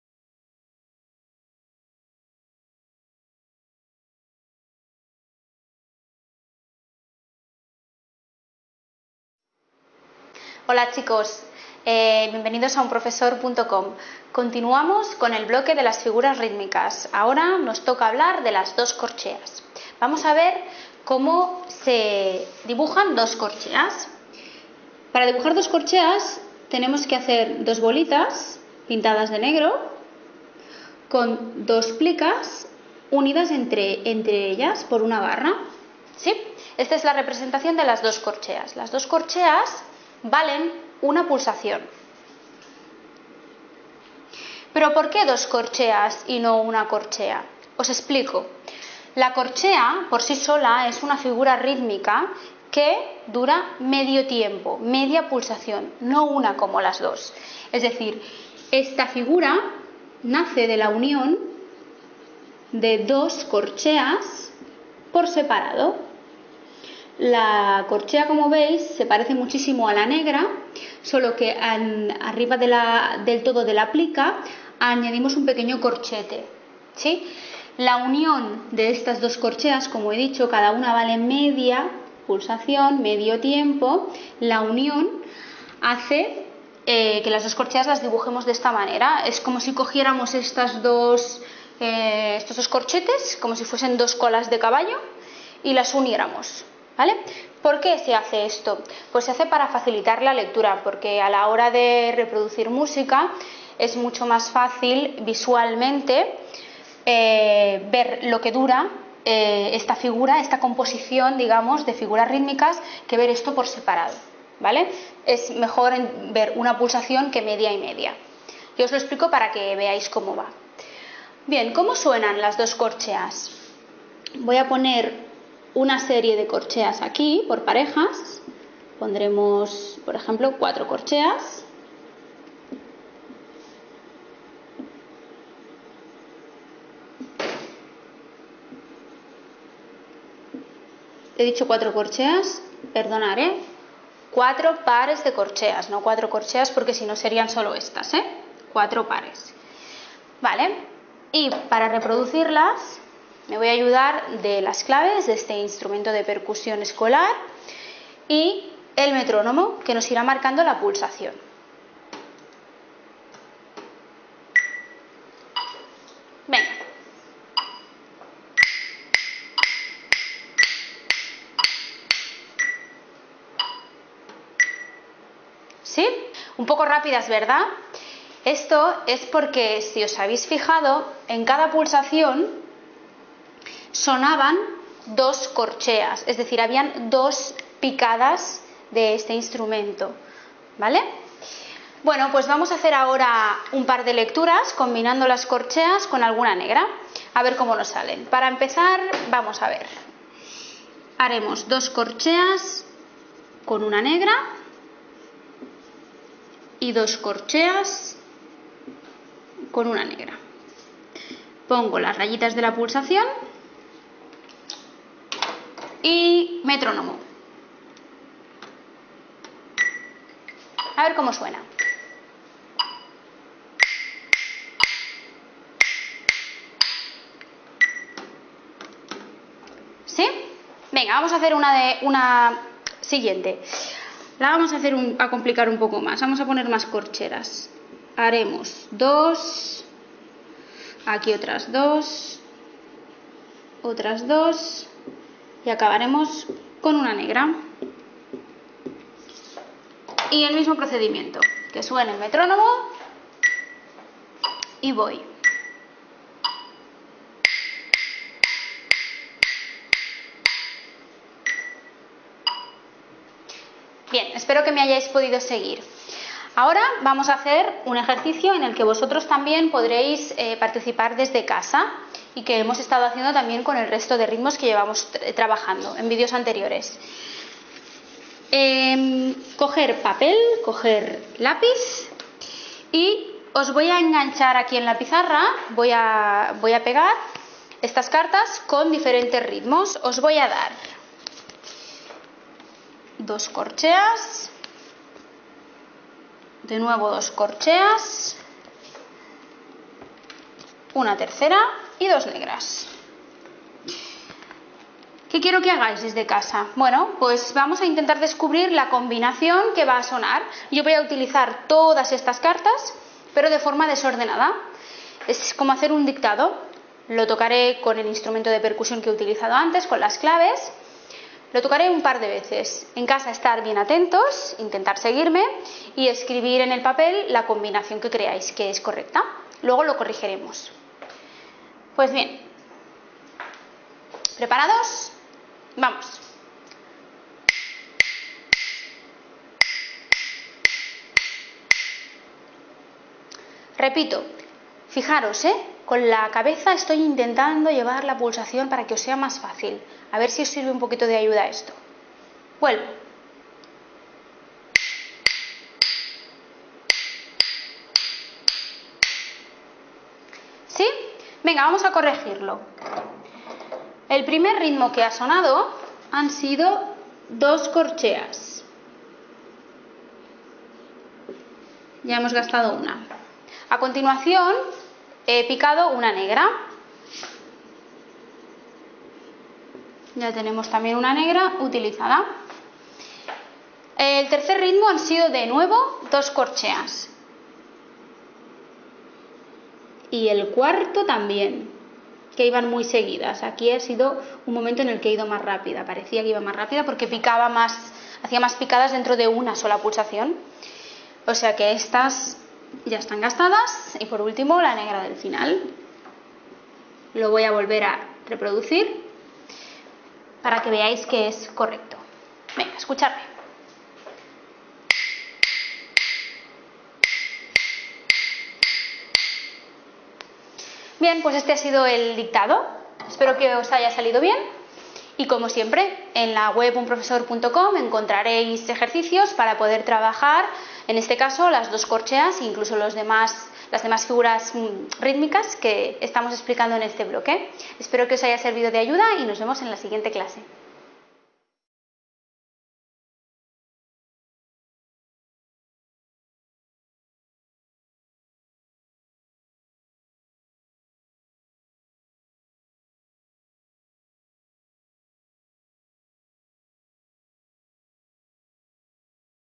Las dos corcheas unidas
Las dos corcheas unidas . Si quieres practicar lo que has aprendido en este vídeo puedes descargarte ejercicios con sus soluciones - Además podrás hacer preguntas al profesor que ha hecho el vídeo.